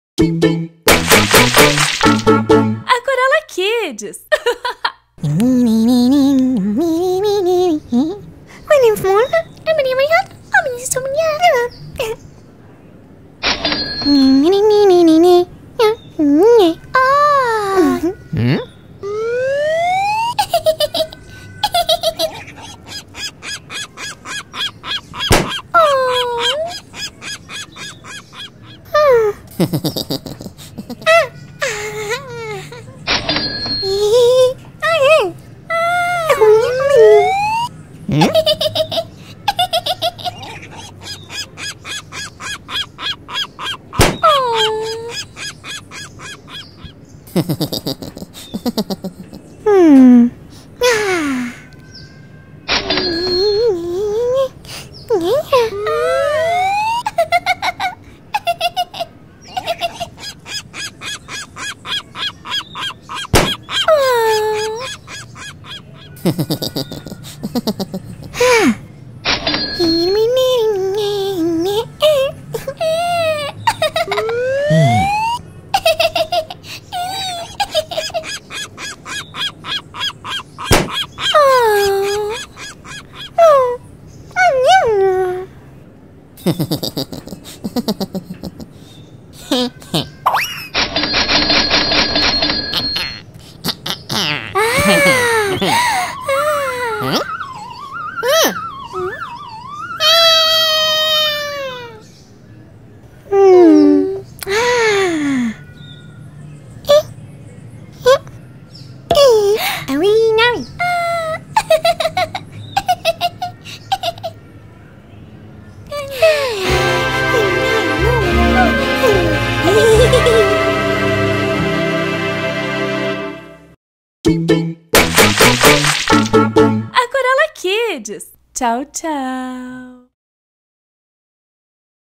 DIA DOS NAMORADOS - O BALÃO
Os personagens do Aquarela Kids estão comemorando o dia dos namorados. Mas nem todos estão felizes. Assista e confira esse vídeo especial dia dos namorados.